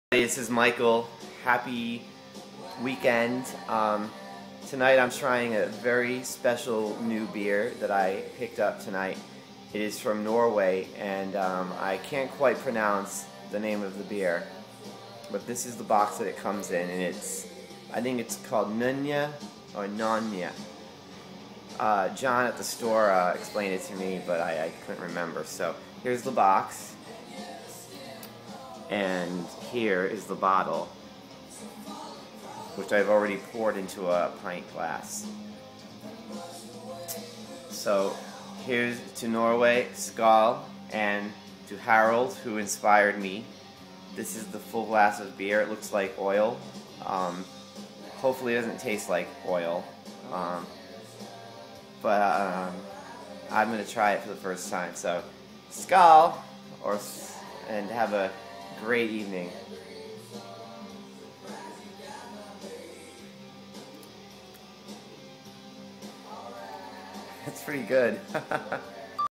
Nogne - Dark Horizon 4.edition
Trying Nogne for the very first time!